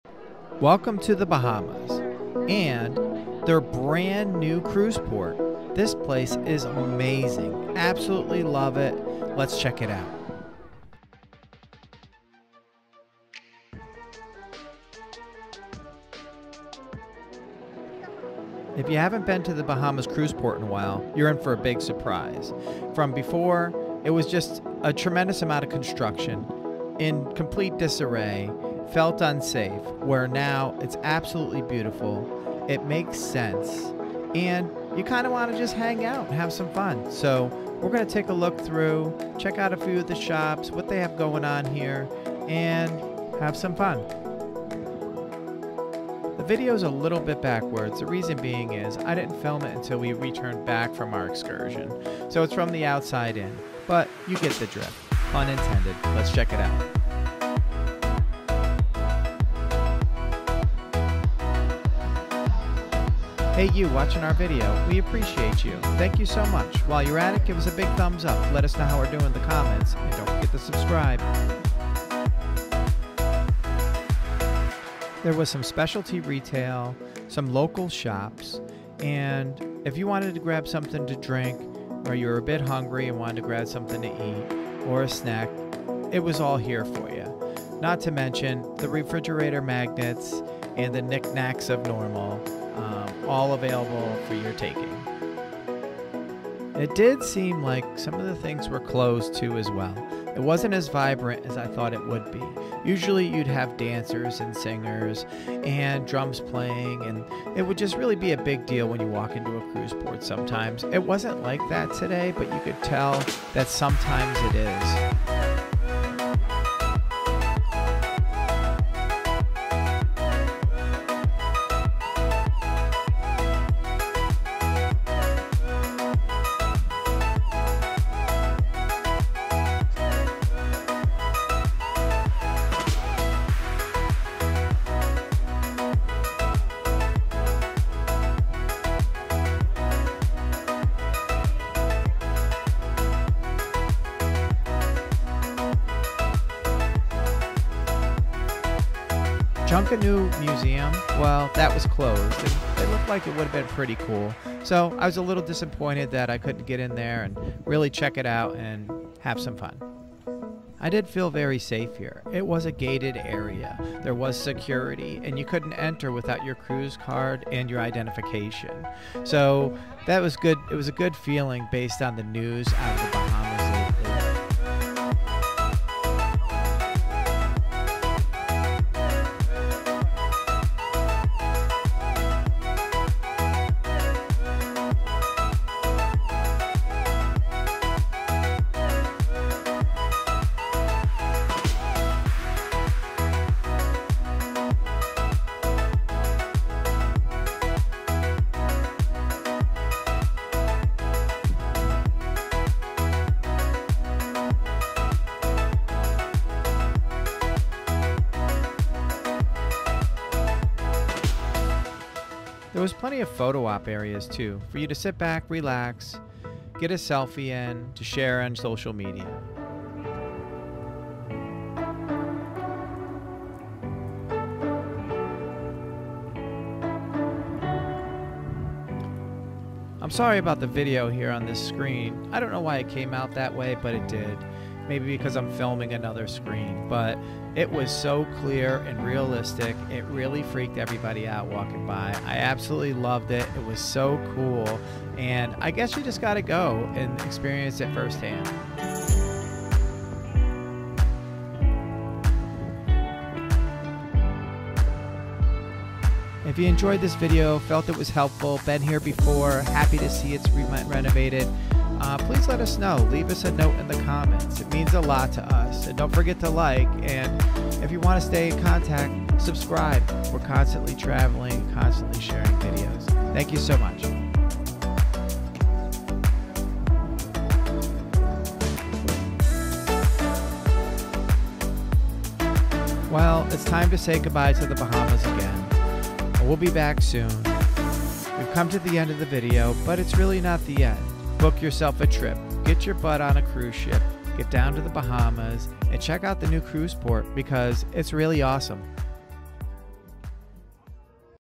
NEW Nassau Bahamas Cruise Port! They did an awesome job!
Let's check out the newly renovated cruise port located in Nassau Bahamas!